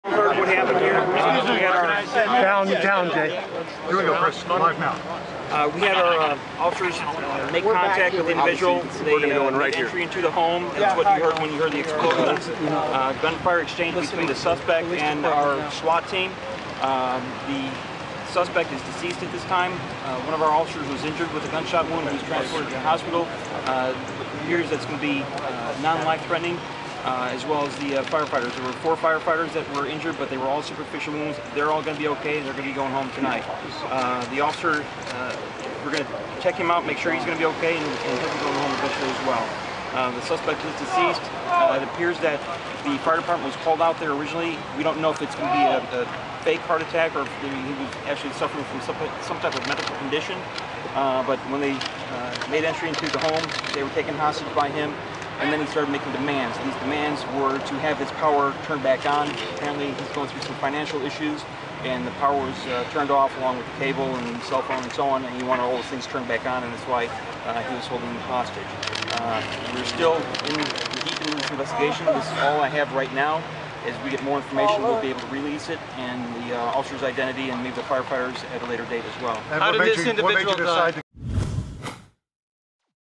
Suwanee SWAT Standoff
A hostage taker is dead after holding several Gwinnett firefighters hostage.